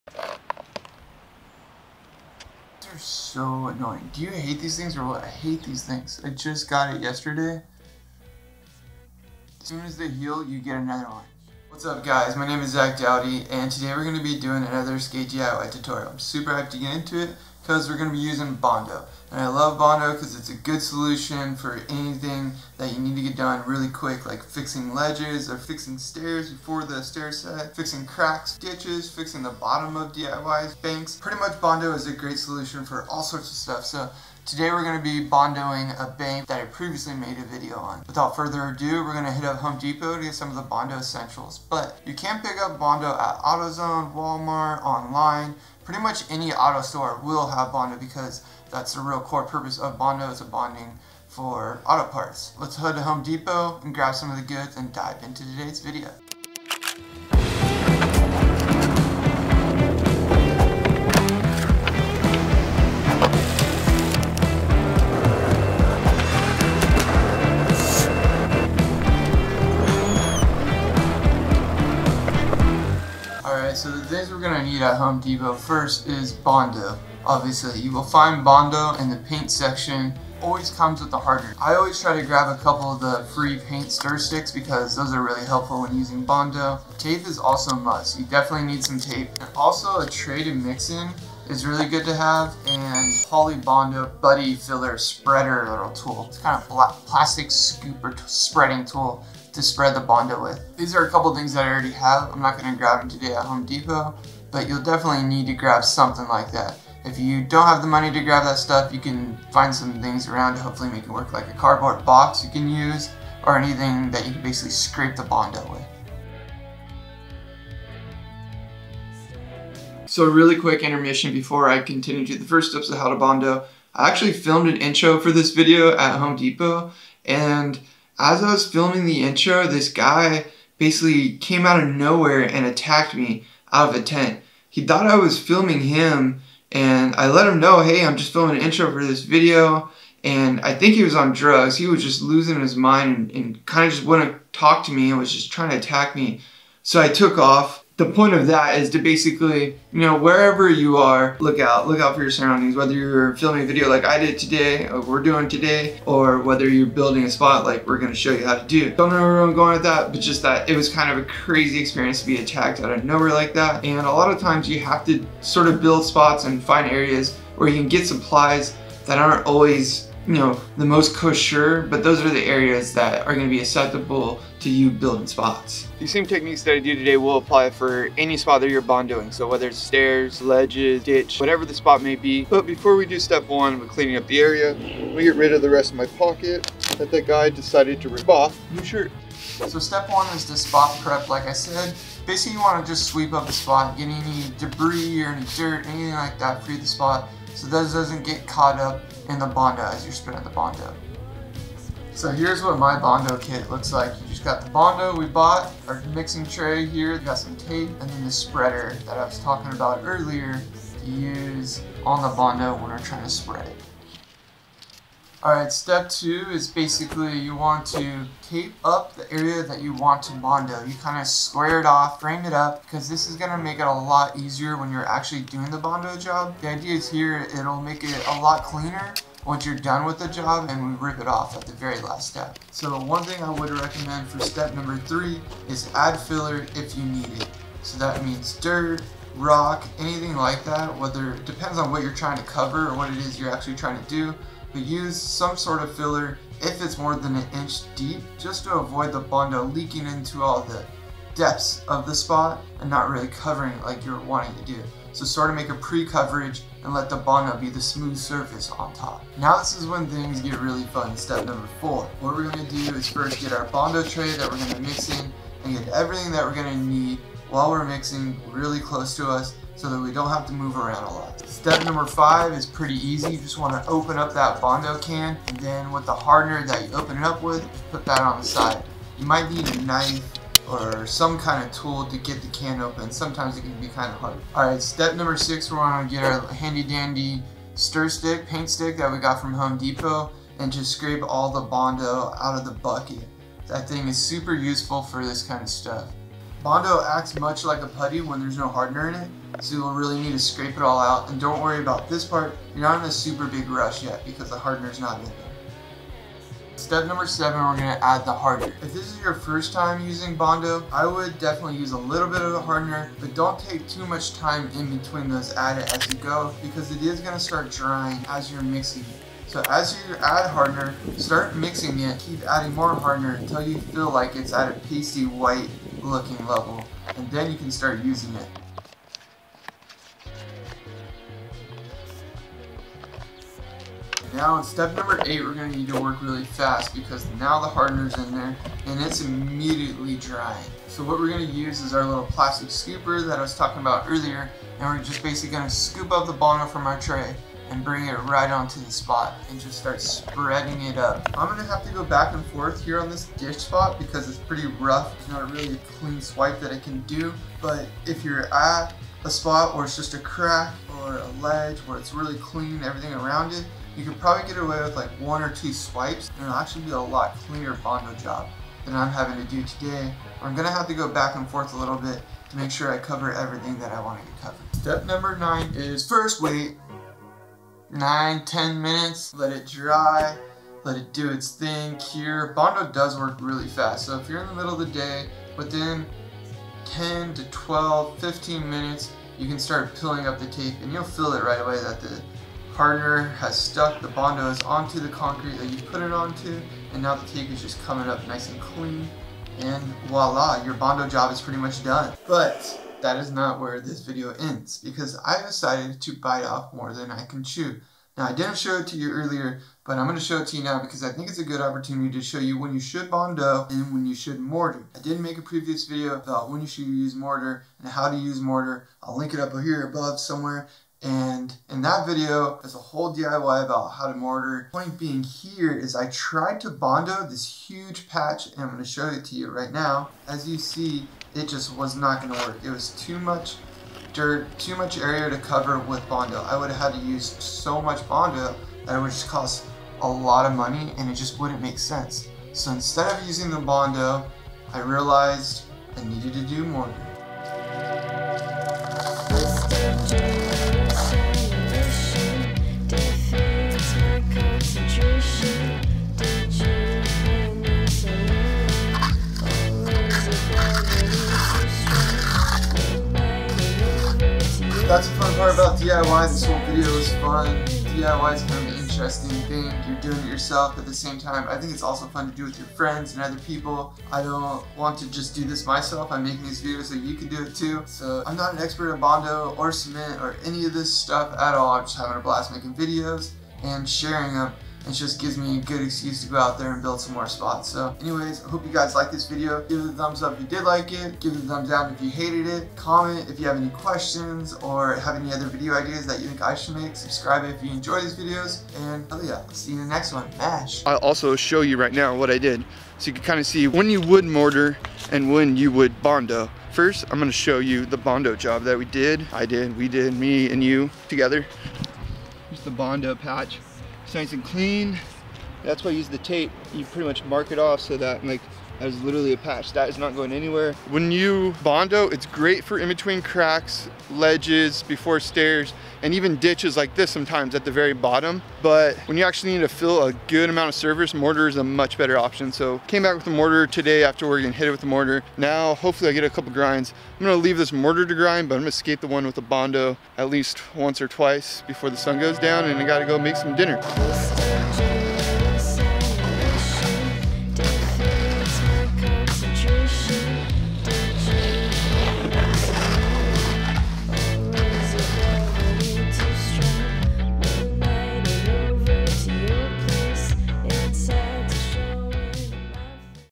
How To Bondo FAILED!?
Going over the basics of How To Bondo a Skate Spot! A nice quick, and easy way to fix any spot.